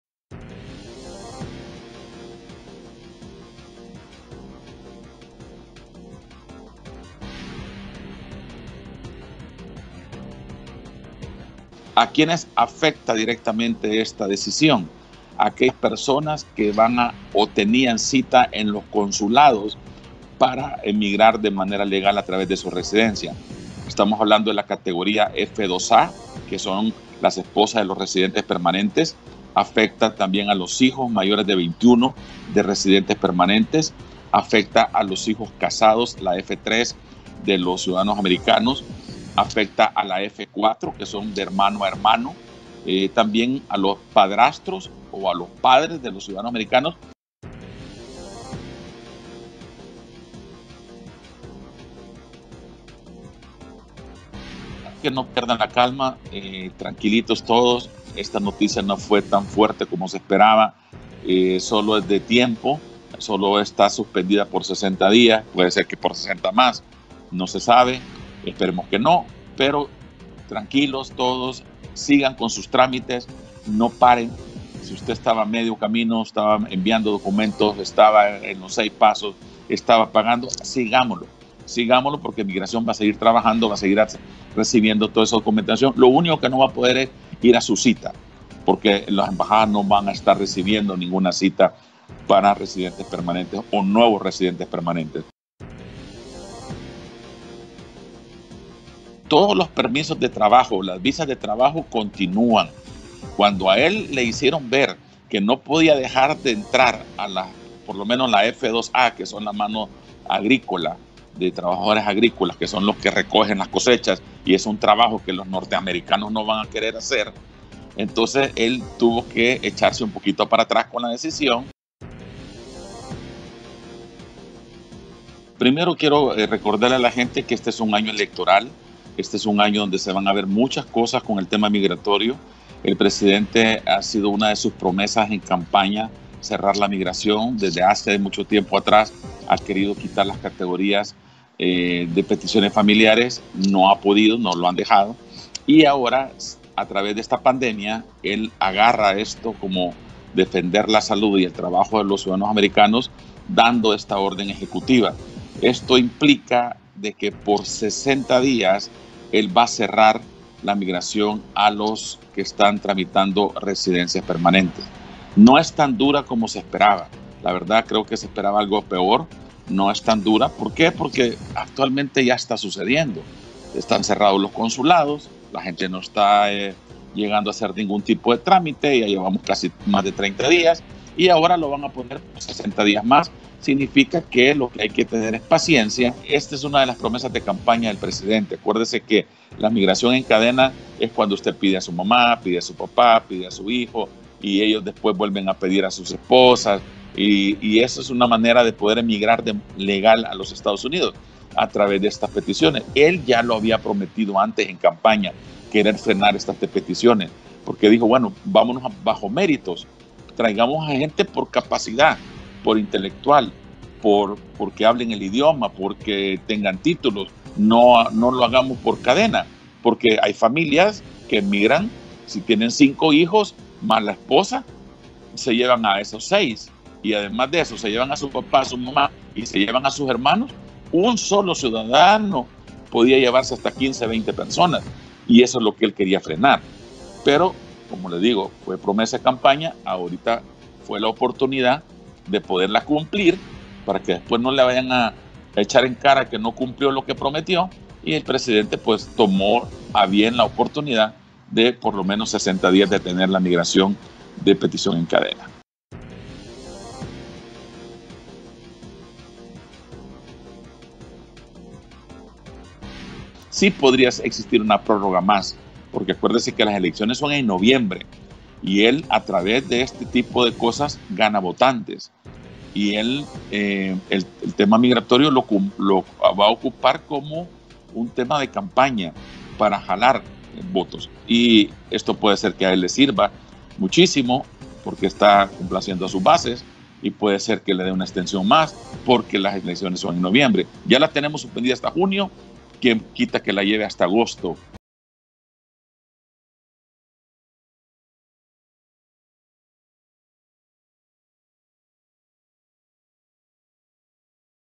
¿A quiénes afecta la suspensión de "green cards" en Estados Unidos?
El experto migratorio Reynaldo Alvergue explica quiénes son los afectados y cómo se debe proceder ante el anuncio dado a conocer esta semana por parte de la administración Trump

¡Visítenos!